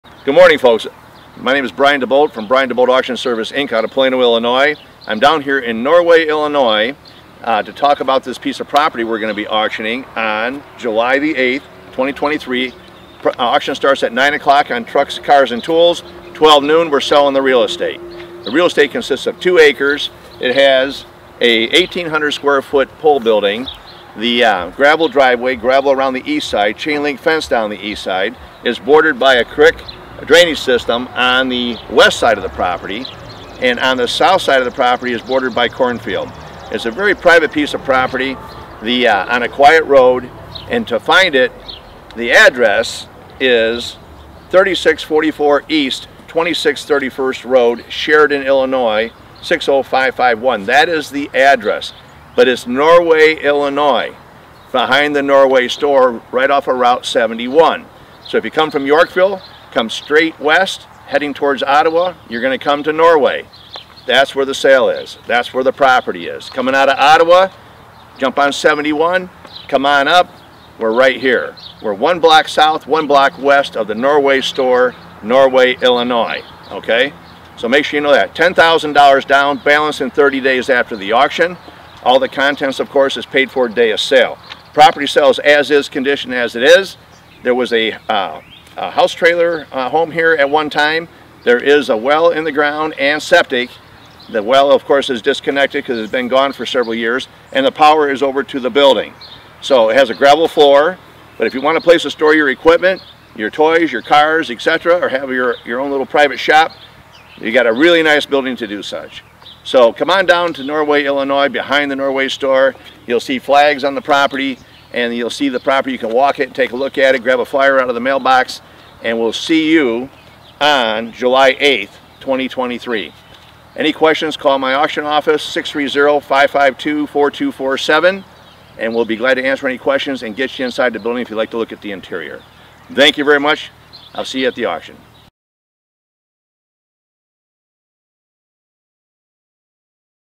PUBLIC REAL ESTATE AUCTION Saturday, July 8th, 2023 9AM Start
PUBLIC REAL ESTATE AUCTION
Saturday, July 8th, 2023  9AM Start
3644 E. 2631st Rd., Sheridan, IL 60551
Directions: Take Rt 71 to Norway, IL.
1 block South West of The Norway Store.
Two Auction Rings!
Real Estate, Cars, Trucks, Tools,
Landscape Equipment, & Much More

Real Estate: 2 Acre Property with creek, Well & Septic, 1,800 Sq. Ft. Pole Barn -
      Two sliding doors and one man door - 35’ x 60’ Cleary Building

1977 Ford Ranchero: New Tires - Radial T/A Tires, New Brake Lines, Dual Exhaust, Chrome Wheels,
           V8 4 Barrel, ~66,000 miles
 1993 Suburban: 186,219 Miles, Auto Trans, 2500 Series, 350 Engine, 4WD, Wester Snow Plow
1966 Ford Fairlane: Hardtop, Auto, 2 Door, V8 Engine w/ most of the parts, hood, Fenders, Trunk Lid, Engine & Trans,  Doors - Garage Kept.
 1994 Chevy S10: 4x4, Automatic Trans, Short Be, Flare Fenders, Side Step, Chrome Rail Bed, has Grill & Parts
 1970 Chevy 1 Ton Truck: Duals, 3spd Stick
 1979 El Camino
 1998 Cadillac Seville
 1993 Chevy S10: 3spd Pickup Truck
 1970 Cargo Truck: w/ Webco Pacific Cargo Box, Auto Trans
 8N Ford Tractor
 Case 646 Loader: Small Utility Tractor w/ Bucket
 Exmark Mower
18’ Flat Bed Trailer: w/ Beavertail, Pinal Hitch & Ramps, Dual Wheels, Side Skidster Forks Holder
Allis Chalmers Small  Utility Tractor

Owner: Juan Rodriguez
Content Terms: No Buyers Premium! No Reserve! Absolute Auction! Cash or good check w/ proper ID. Everything sold as is. Everything paid for day of sale. Not responsible for accidents or items after sold. All Equipment and Material Sells AS IS Condition Day of Sale.  No Reserve! Everything Sells. Please try to attend this quality sale. Many items not listed at this time. This is a Partial Listing Only. Announcements day of sale supersede communication and printed materials. We accept Credit Cards! 4% Electronic Transaction Fee for On-Site Bidders. Fees/Terms may vary for remote and online bidders.

Brian DeBolt Auction Service, Inc. Since 1987